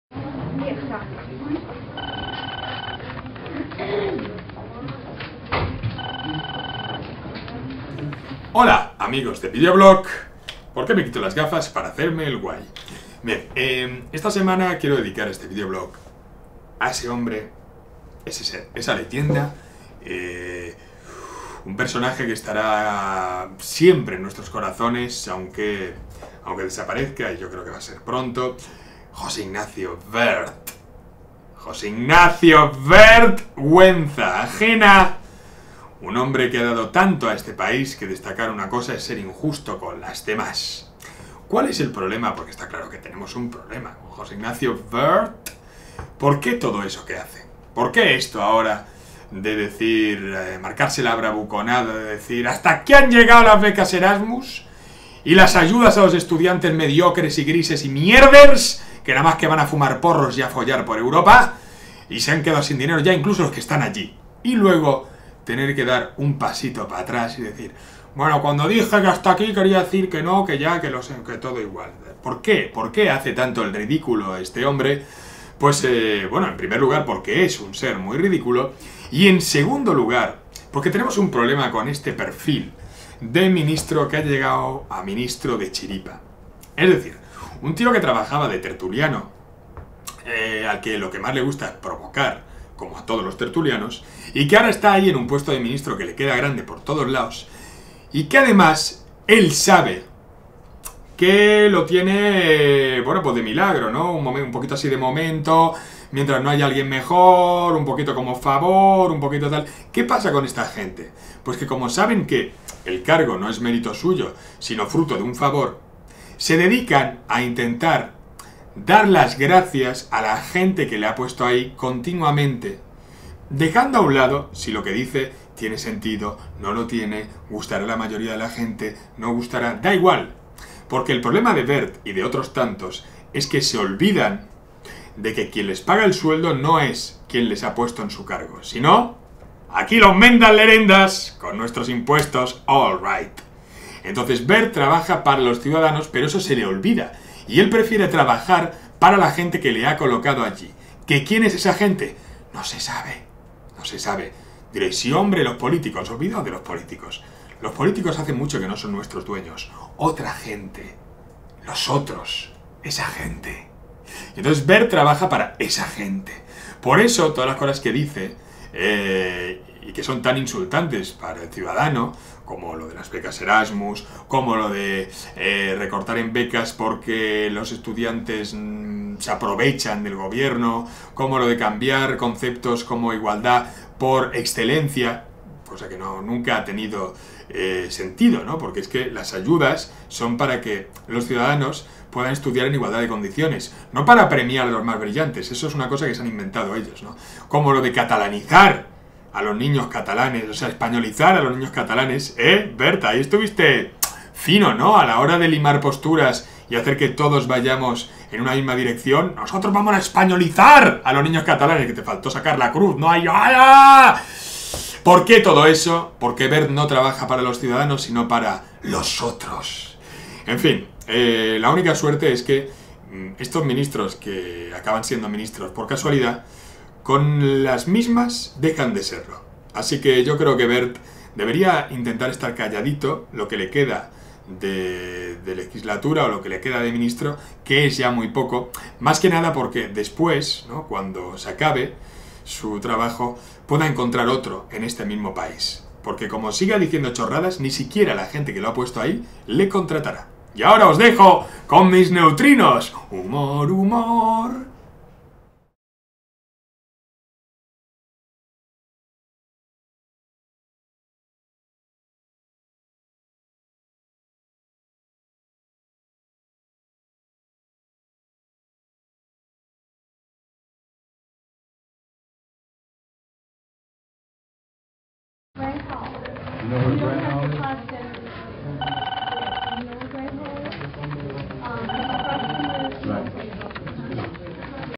Dani Mateo analiza la imagen de José Ignacio Wert
Dani Mateo analiza la figura del ministro de Educación, José Ignacio Wert.